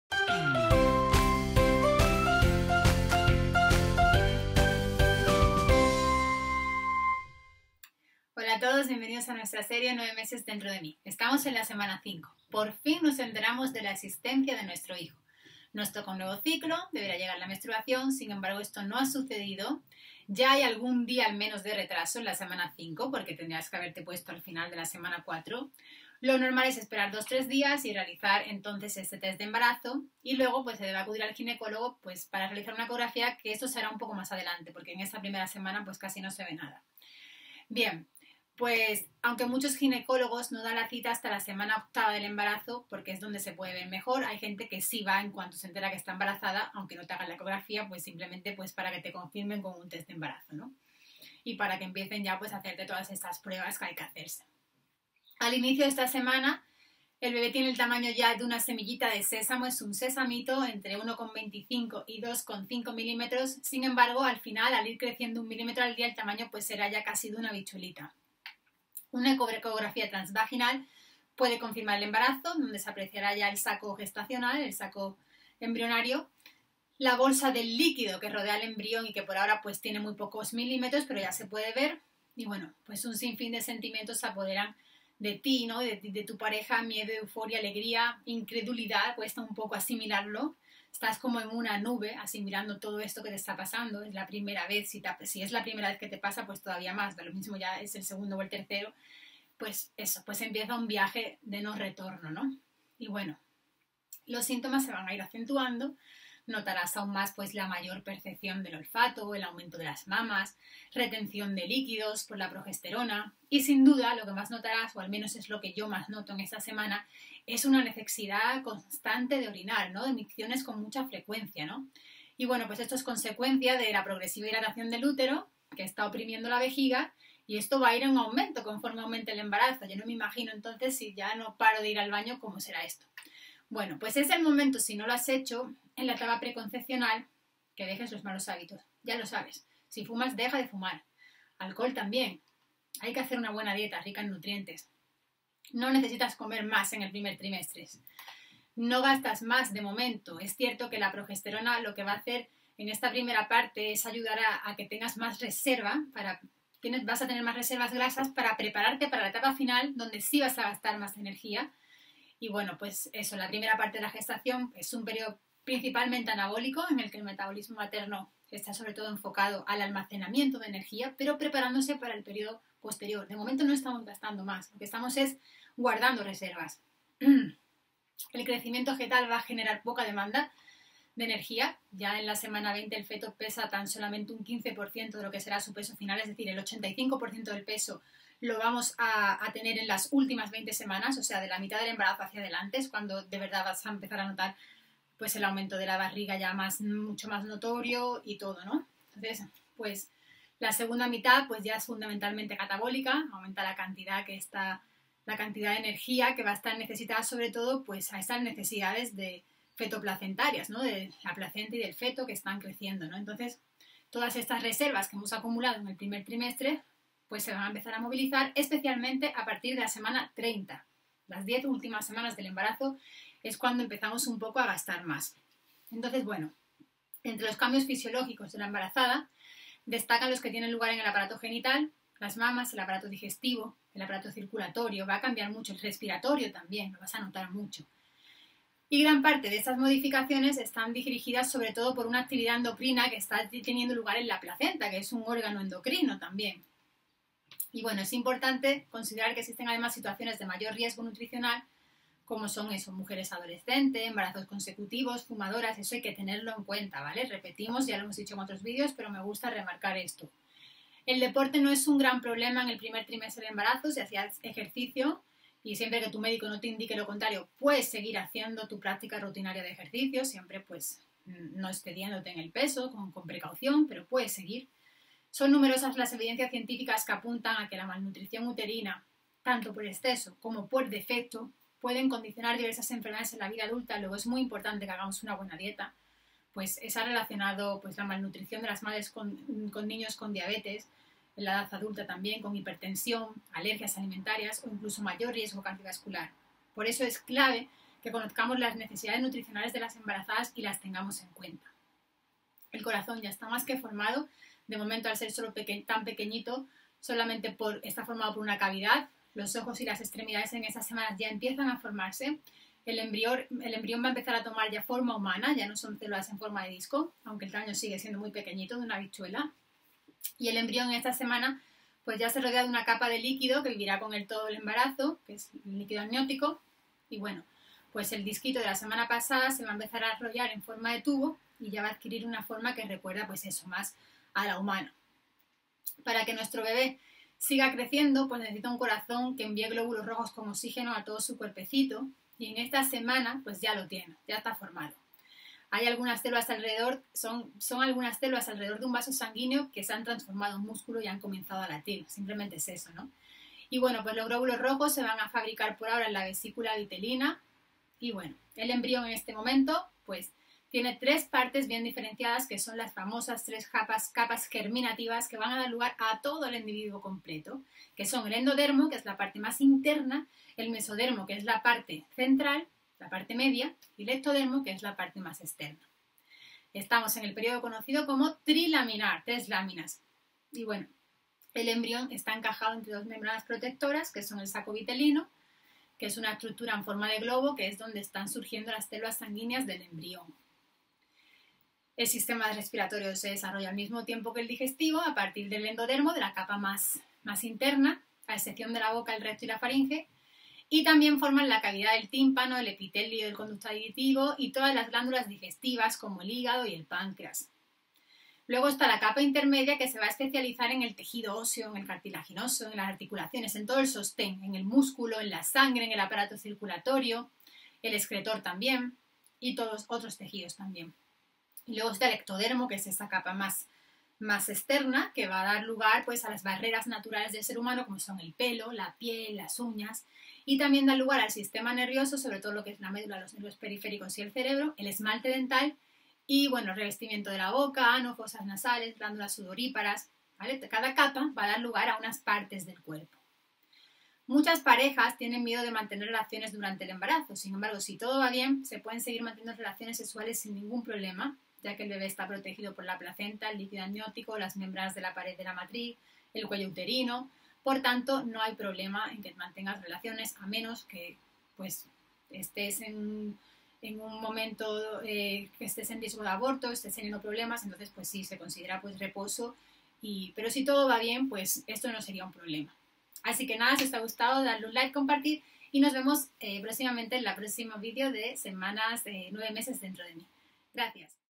Semana 5 de Embarazo.
En este vídeo hablaremos de la quinta semana de embarazo. El bebé tiene el tamaño de una semillita de sésamo y ya sabemos qué estamos esperando un bebé. Hablaremos de las necesidades energéticas en cada etapa del embarazo.